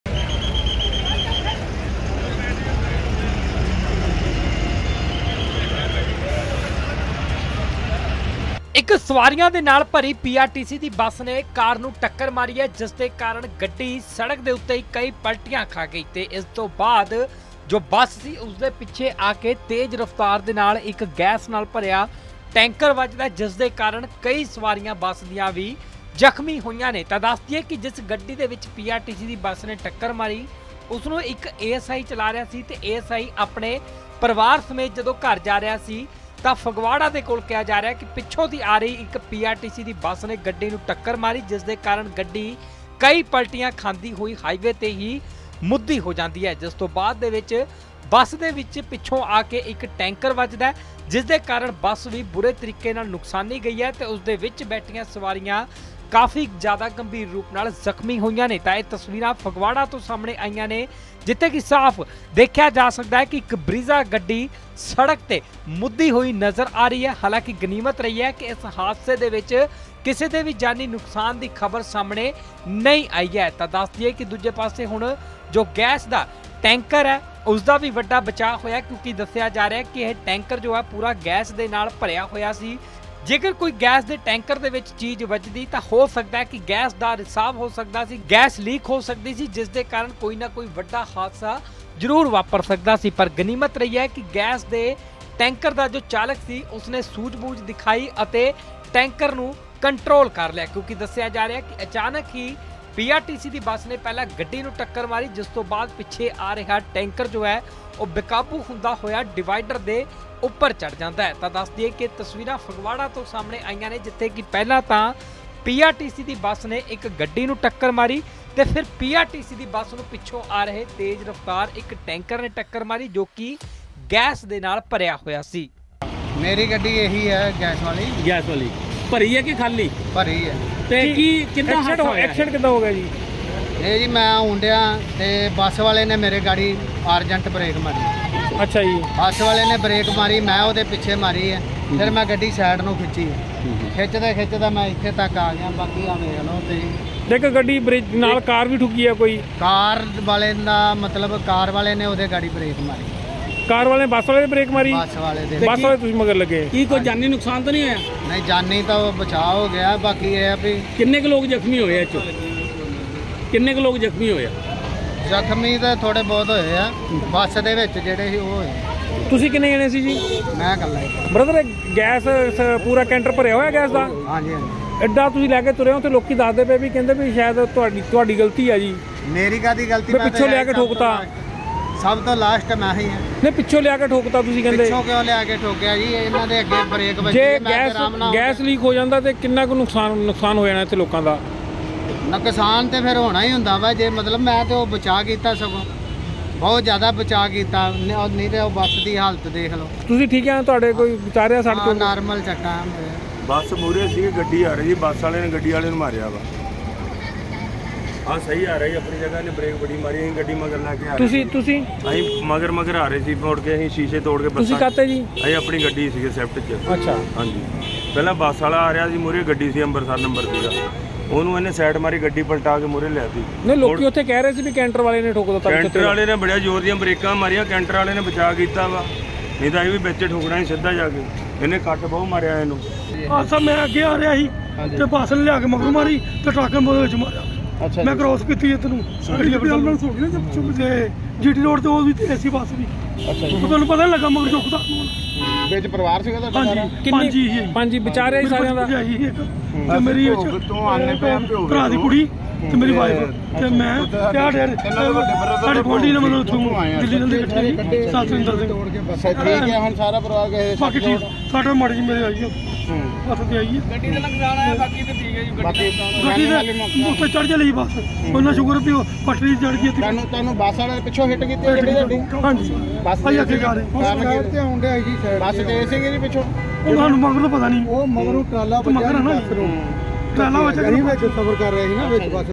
PRTC ਦੀ ਬੱਸ ਨੇ ਮਾਰੀ ASI ਦੀ ਕਾਰ ਨੂੰ ਟੱਕਰ, ਹਾਈਵੇਅ 'ਤੇ ਲੱਗਿਆ ਲੰਬਾ ਜ਼ਾਮ | Phagwara Accident | N18V
PRTC ਦੀ ਬੱਸ ਤੇ ਕਾਰ ਵਿਚਕਾਰ ਹੋਈ ਟੱਕਰ। ਹਾਈਵੇਅ ਤੇ ਪਲਟ ਗਈ Brezza ਗੱਡੀ। ਬੱਸ ਦੇ ਪਿੱਛੋਂ ਵੱਜਿਆ ਗੈਸ ਨਾਲ ਭਰਿਆ ਟੈਂਕਰ। ਹਾਈਵੇਅ ਤੇ ਲੱਗਿਆ ਗੱਡੀਆਂ ਦਾ ਲੰਬਾ ਜ਼ਾਮ।